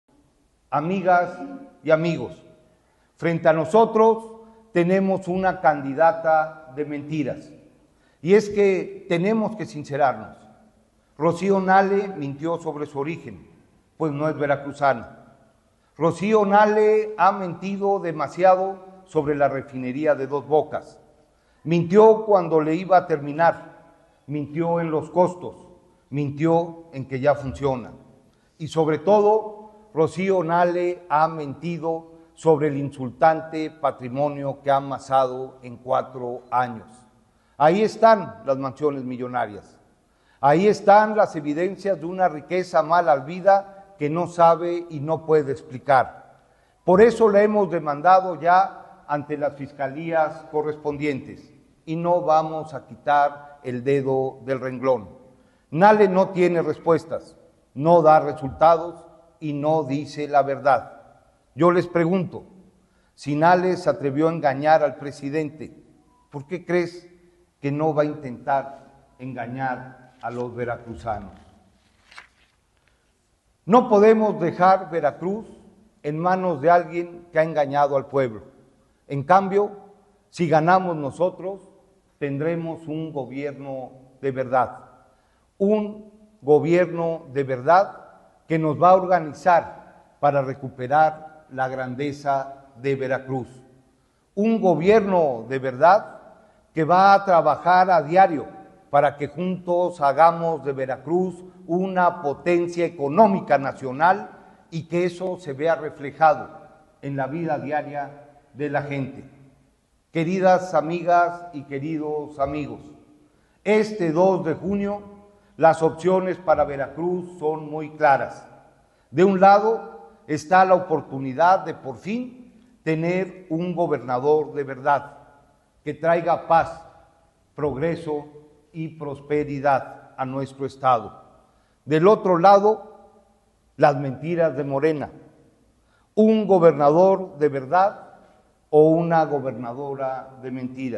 Conferencia de prensa del Candidato José Yunes Zorrilla
En el arranque de campaña de Pepe Yunes, el candidato presentó 10 propuestas para que Veracruz tenga un Gobernador de Verdad y recupere su grandeza.
Vamos a mantener, ampliar y mejorar los programas sociales, aseguró.